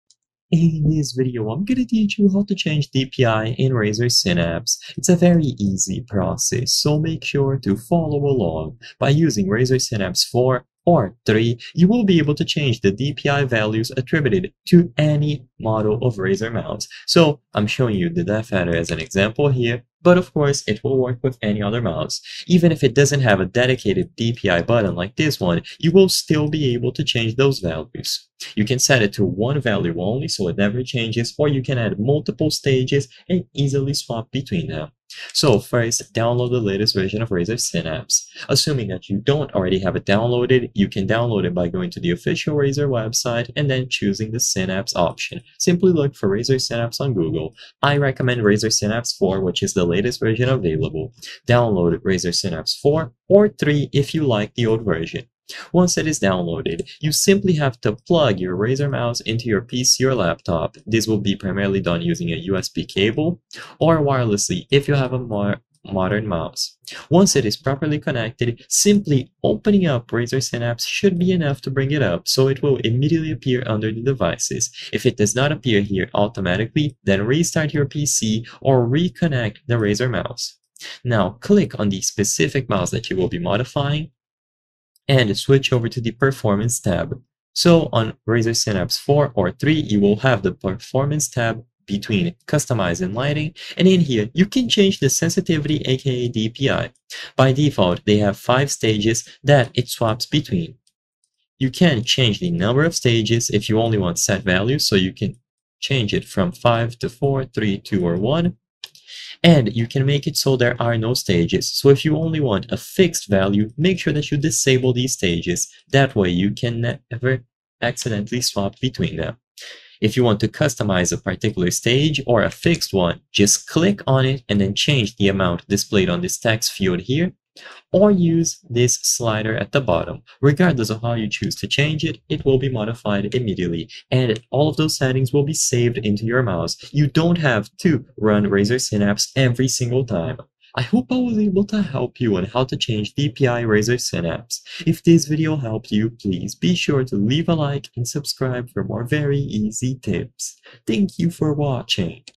How To Change DPI Razer Synapse (2025) - Step by Step
I will solve your doubts about how to change dpi razer synapse, and whether or not it is possible to do this.

I hope I have helped you with the video!